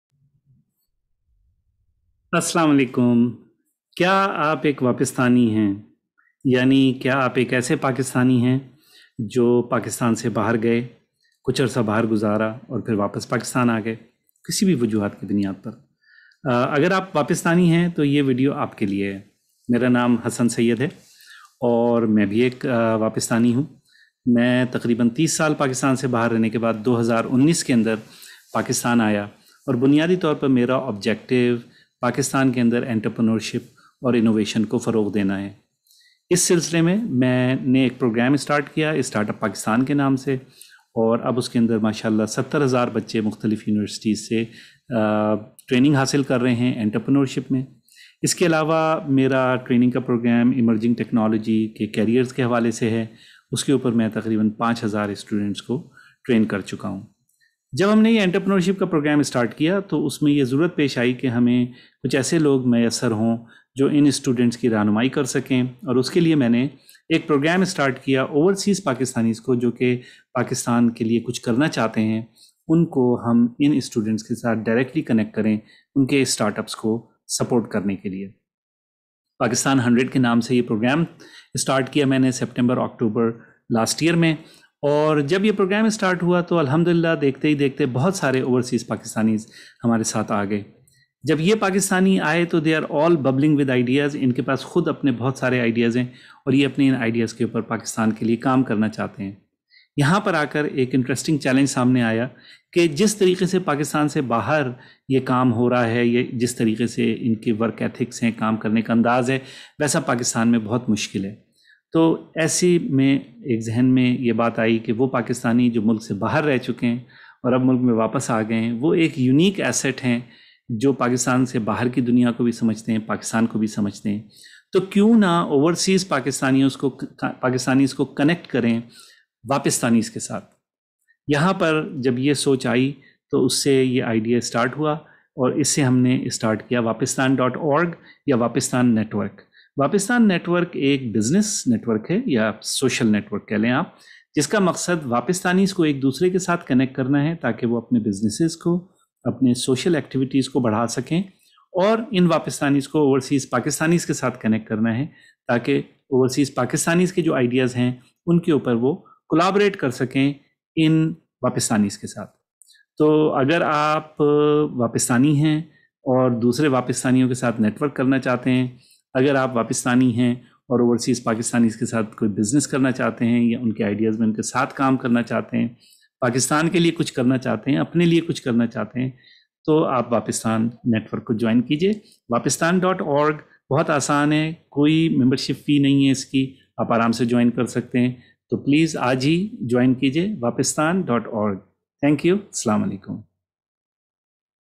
Why Wapistan Network? Join Today
and click on Become a Member button.

Wapistanis are a great intellectual asset for Pakistan. Wapistanis could contribute towards a prosperous Pakistan if they could join forces internally with each other and with Overseas Pakistanis.

The purpose of this community is to connect Wapistanis and Overseas Pakistanis to create value for the members and for Pakistan.